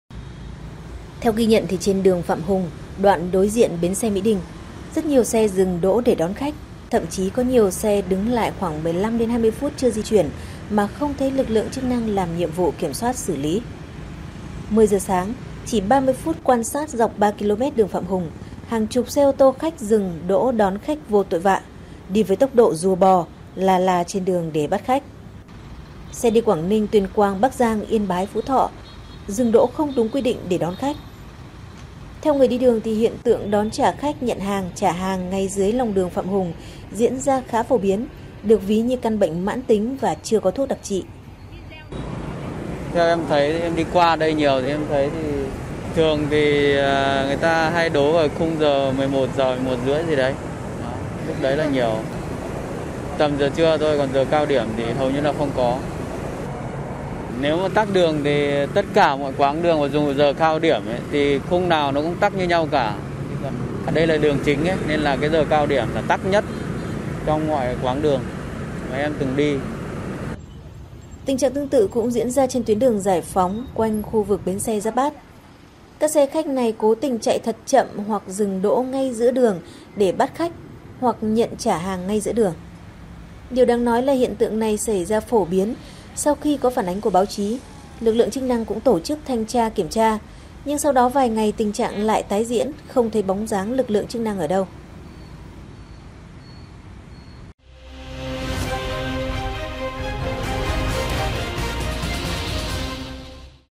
Tái diễn đón trả khách dọc đường Giải Phóng, Phạm Hùng Cơ quan chức năng ở đâu?
Hiện tượng đón, trả khách, nhận hàng, trả hàng ngay dưới lòng đường Phạm Hùng, Giải Phóng diễn ra khá phổ biến. Điều đáng nói là sau khi có phản ánh của báo chí, lực lượng chức năng tổ chức thanh tra, kiểm tra nhưng sau đó vài ngày tình trạng lại tái diễn mà không thấy lực lượng chức năng

37 Bà Triệu, Hoàn Kiếm, Hà Nội, Việt Nam